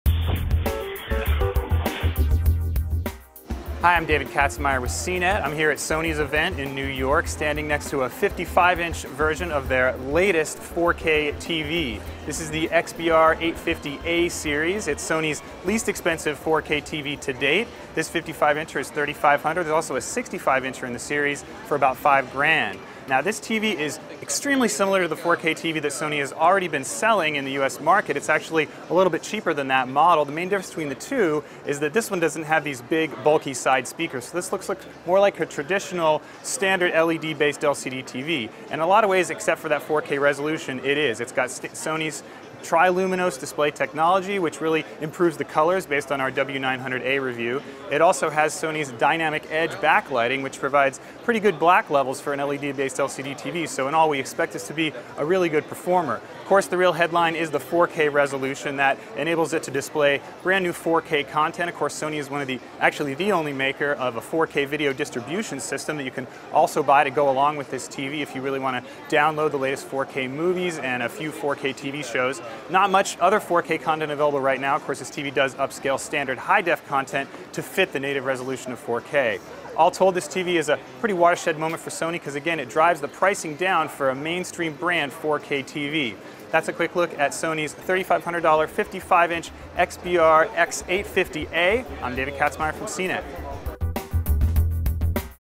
Sony XBR-X850A series: 4K TV gets (slightly) cheaper
Sony's newest 4K TV is also the least-expensive yet from a major TV maker, starting at $3500 for the 55-inch size.